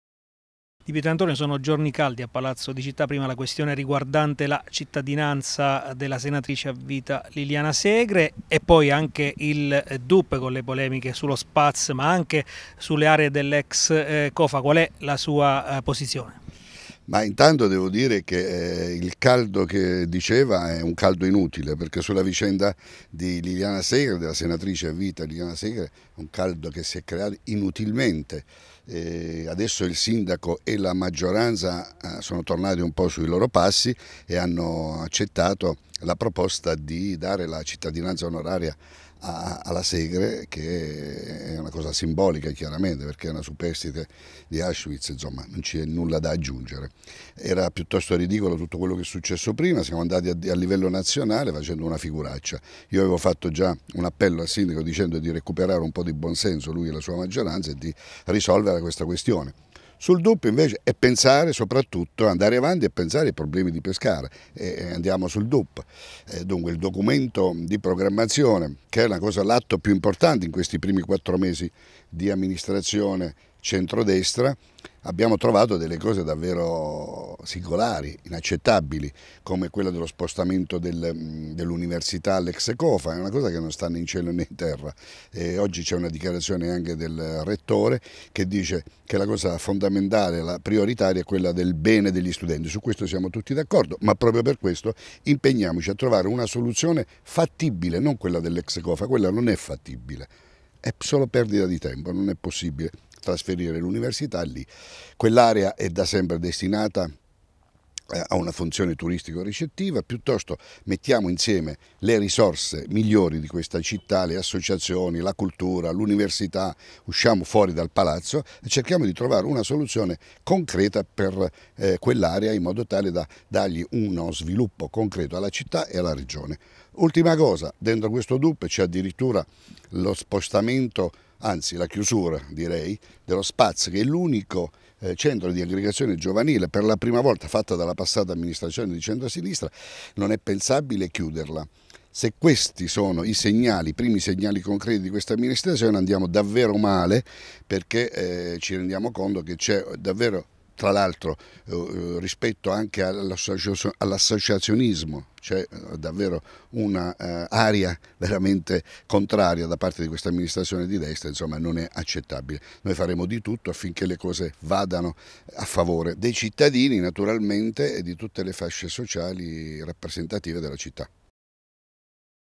Di Pietrantonio critica la Giunta Masci sul caso Segre e Dup
Sulle polemiche riguardanti la cittadinanza onoraria di Pescara a Liliana Segre e il Dup , interviene il segretario cittadino del Pd Moreno Di Pietrantonio che critica la Giunta Masci